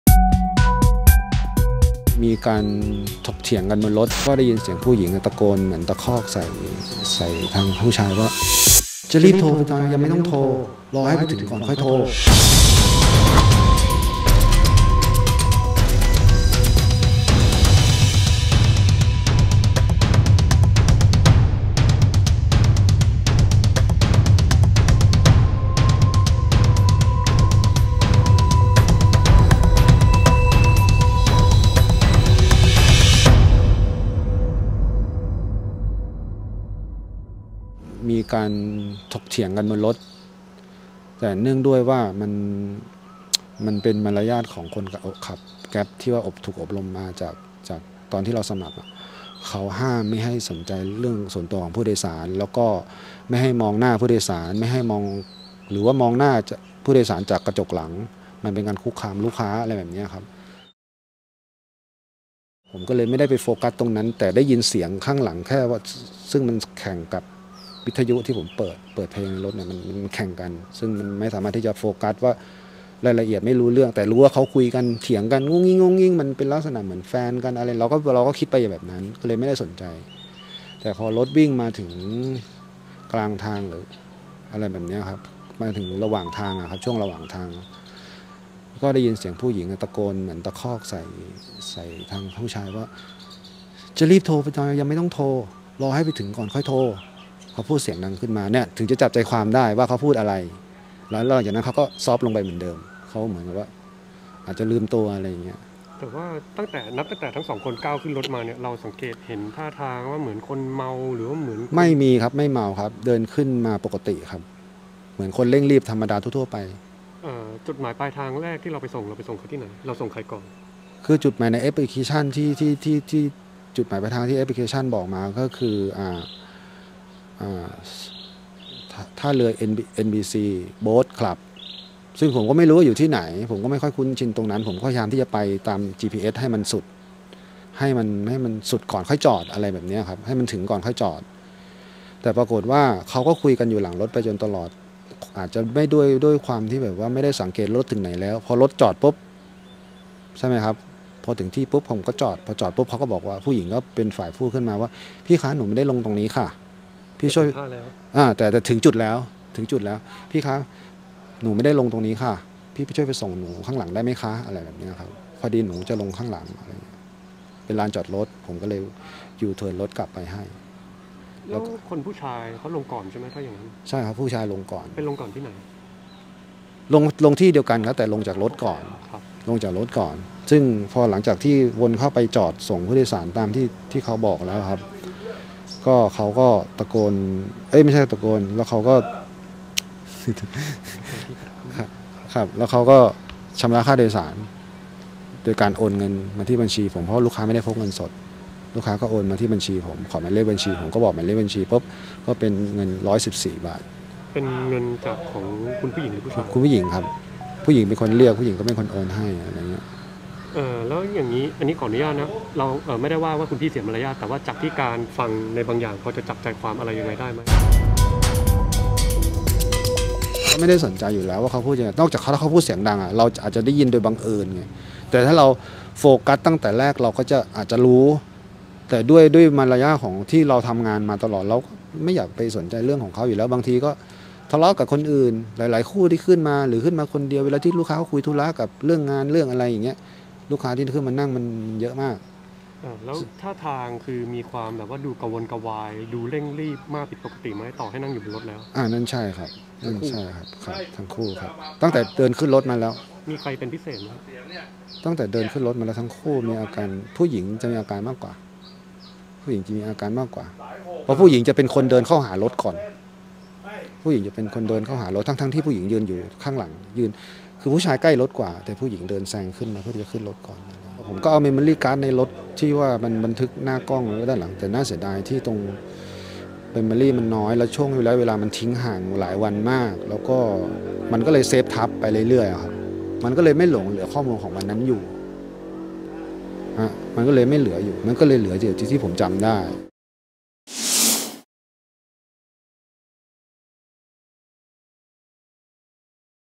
เปิดใจแกร็บ รับกระติก-โรเบิร์ต วันแตงโมจมน้ำ ผู้หญิงตะคอกในรถ!!! : Khaosod TV
เปิดใจแกร็บ รับกระติก-โรเบิร์ต วันแตงโมจมน้ำ ผู้หญิงตะคอกในรถ!!!